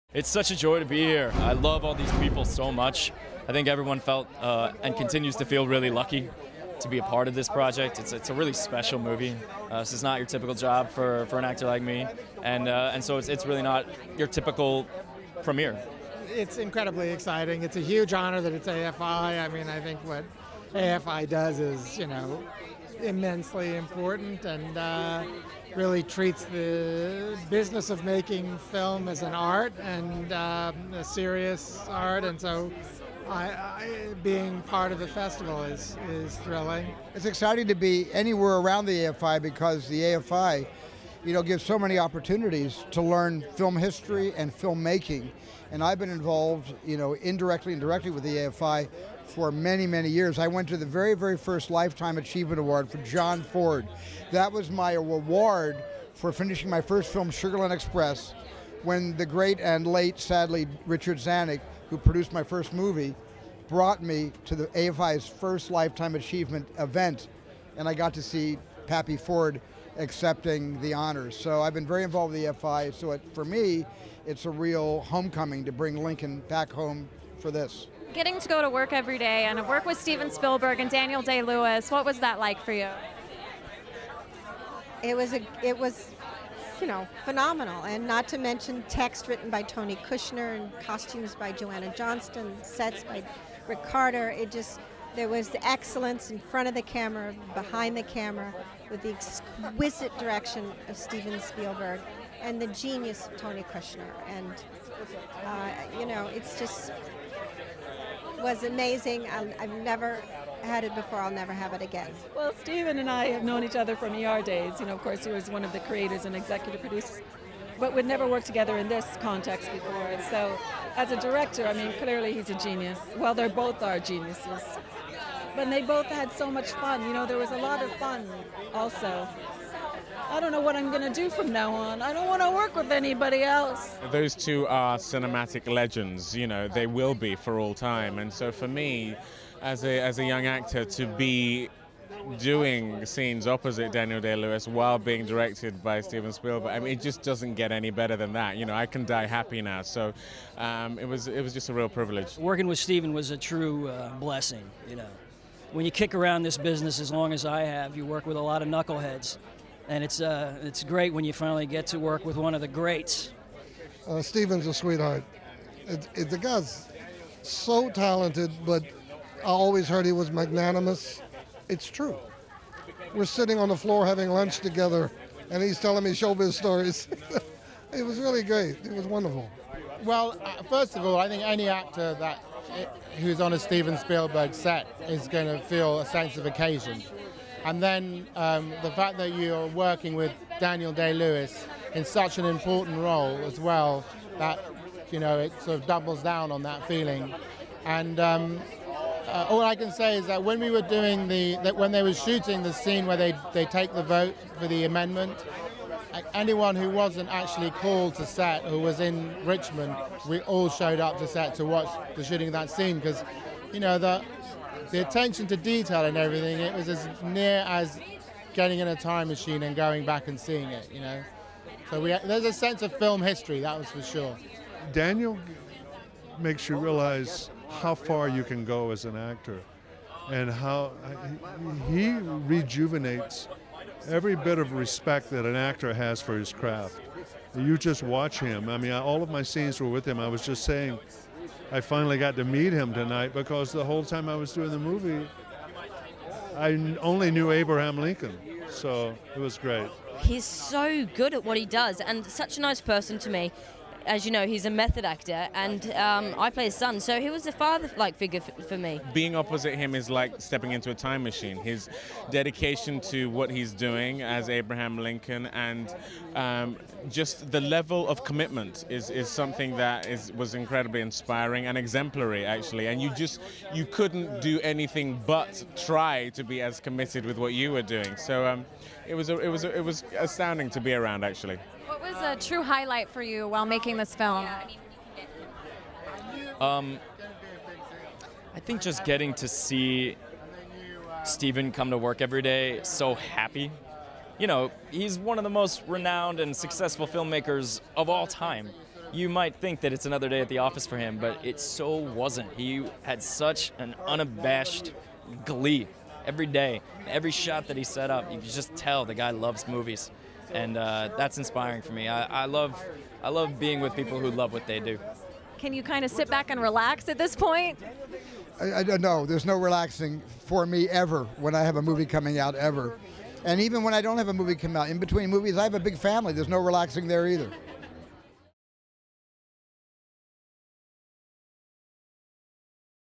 Steven Spielberg, Joseph Gordon-Levitt, Sally Field at the Premiere of LINCOLN at AFI FEST
The cast and crew of LINCOLN talk about what it was like to work on the film at the premiere at AFI FEST presented by Audi.